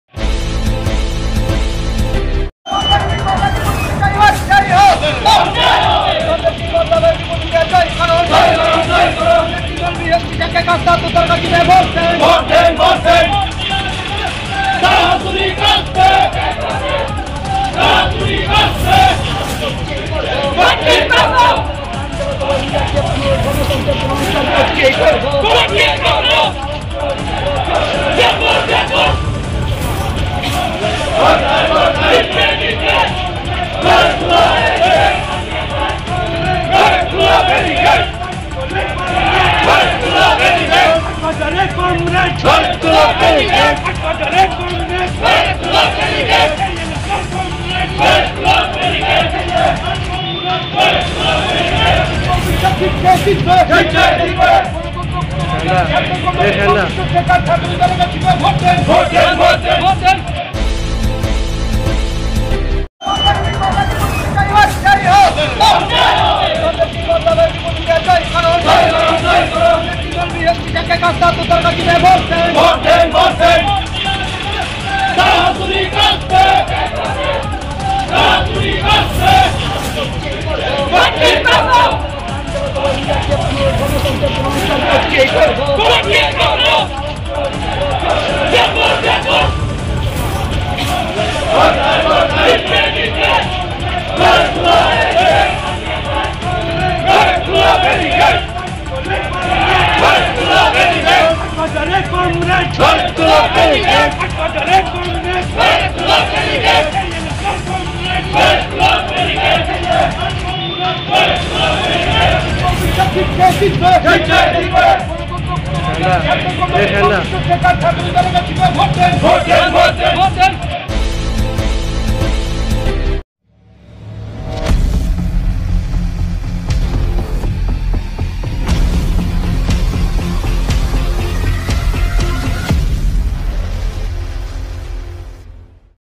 প্রার্থী নির্মল বিশ্বাস এর সমর্থনে খোয়াইতে সাড়া জাগানো মিছিল
আসন্ন ত্রিপুরা বিধানসভা নির্বাচনে ২৫ খোয়াই বিধানসভা কেন্দ্রের বামফ্রন্ট মনোনীত সিপিআই(এম) প্রার্থী কমরেডঃ নির্মল বিশ্বাস এর সমর্থনে বুধবার খোয়াইতে সাড়া জাগানো মিছিল।